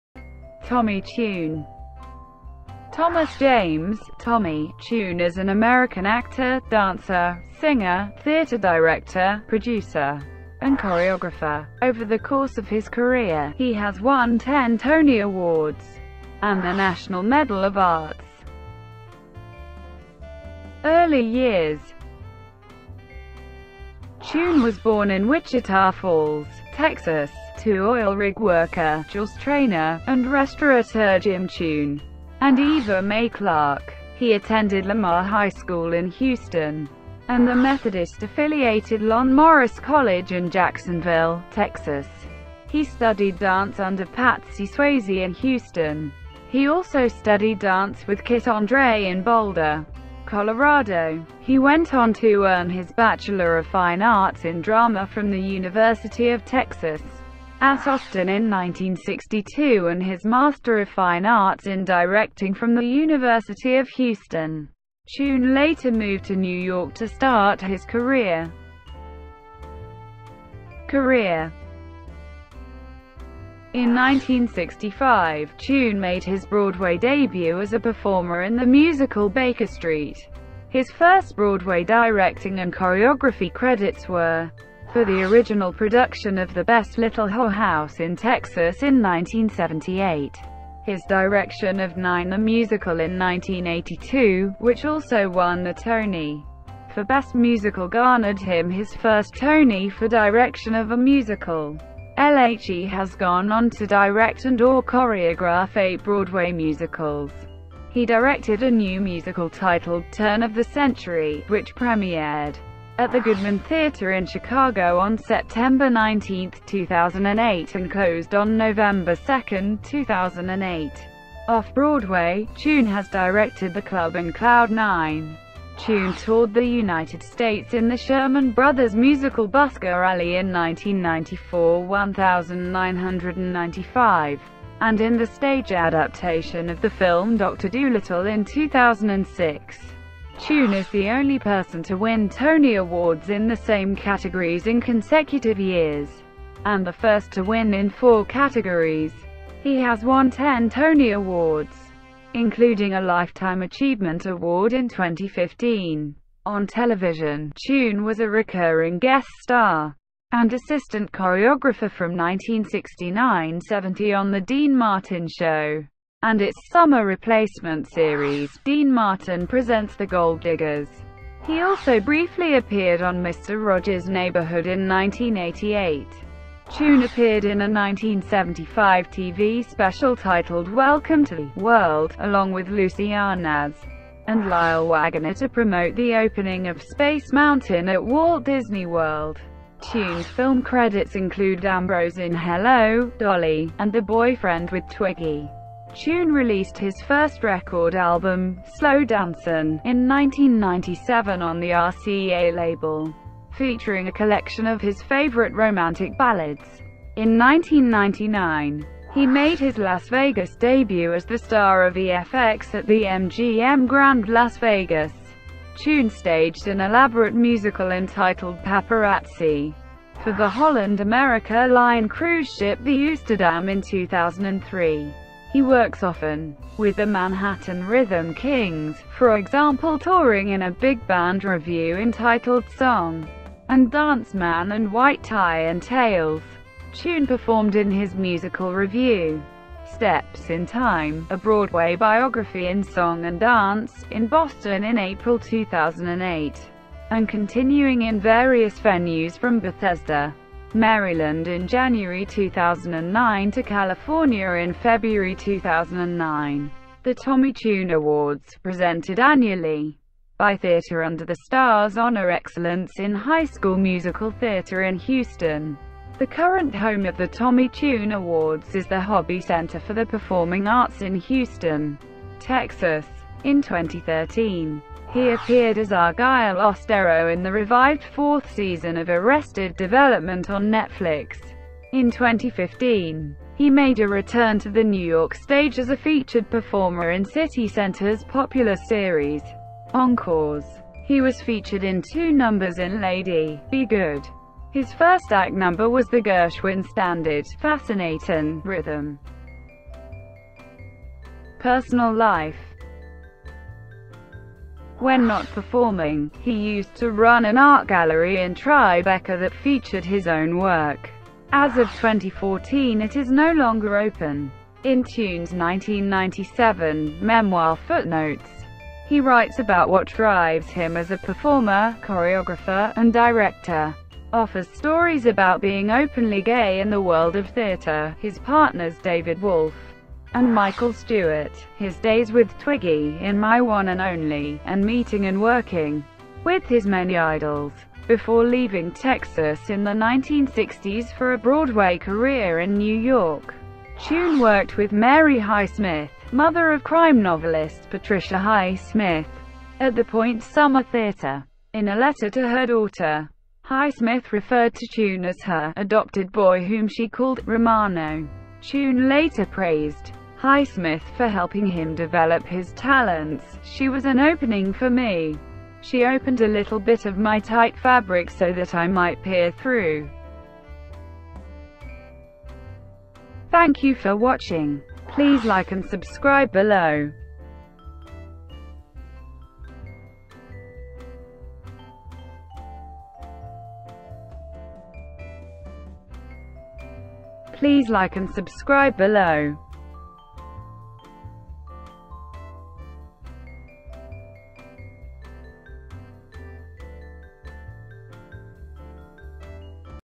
Tommy Tune - WikiVidi Documentary
Thomas James "Tommy" Tune is an American actor, dancer, singer, theatre director, producer, and choreographer. Over the course of his career, he has won ten Tony Awards and the National Medal of Arts.

Shortcuts to chapters:
0m3s+: Early years
0m8s+: Career
3m54s+: Personal life

Copyright WikiVidi.